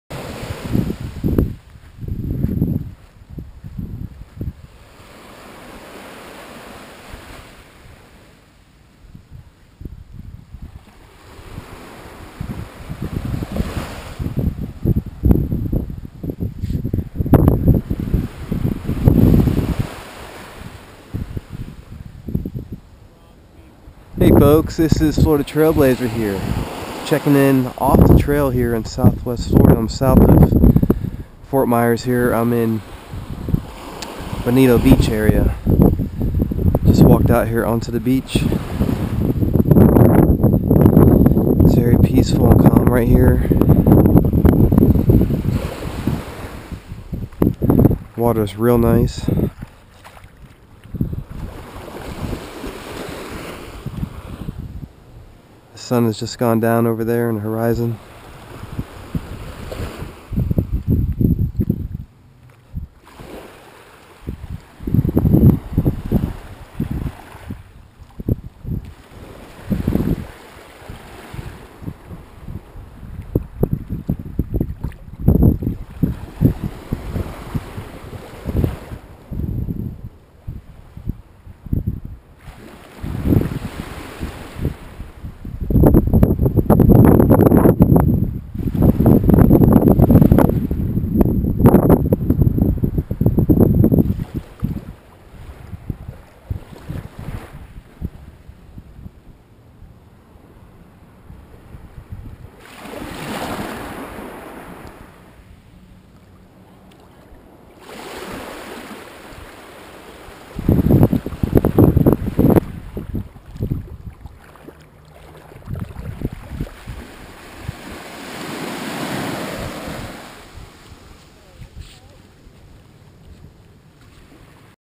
Bonita Beach at Sunset
After a day of exploring I stopped by this beach just around sunset. This is Bonita Beach on the gulf coast side of Florida.